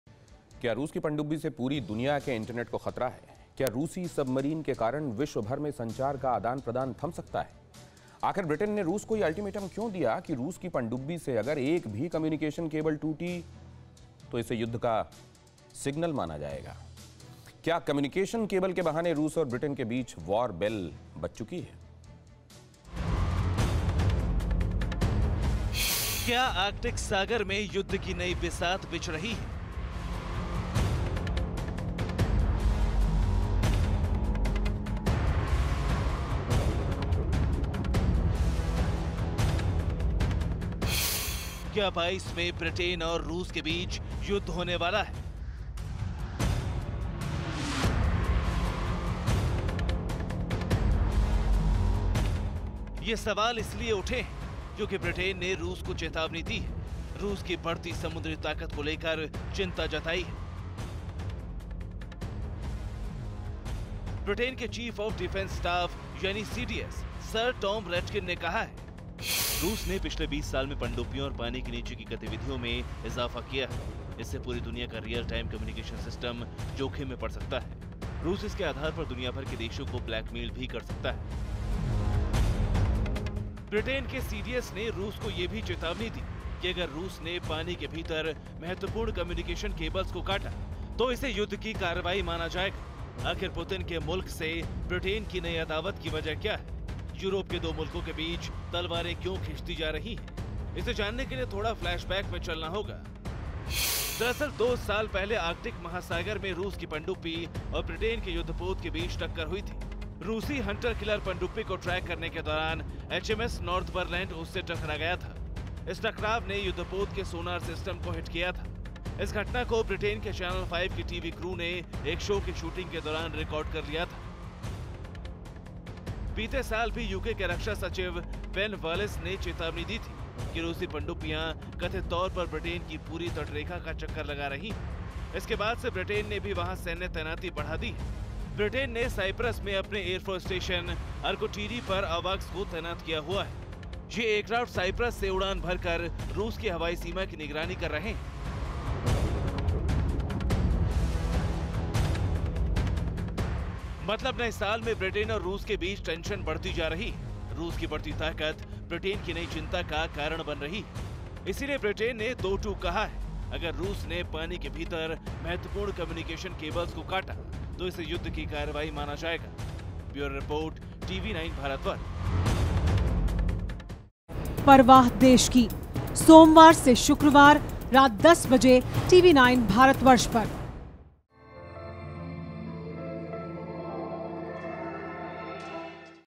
क्या 22 में ब्रिटेन और रूस के बीच युद्ध होने वाला है ? | Britain Russia War
क्या रूस की पनडुब्बी से पूरी दुनिया के इंटरनेट को खतरा है ? क्या रूसी सबमरीन के कारण विश्व भर में संचार का आदान-प्रदान थम सकता है ? आखिर ब्रिटेन ने रूस को ये अल्टीमेटम क्यों दिया कि रूस की पनडुब्बी से अगर एक भी कम्युनिकेशन केबल टूटेगी. तो इसे युद्ध का कवायद माना जाएगा ? क्या कम्युनिकेशन केबल के बहाने रूस और ब्रिटेन के बीच वॉर बेल बज चुकी है ?

Credit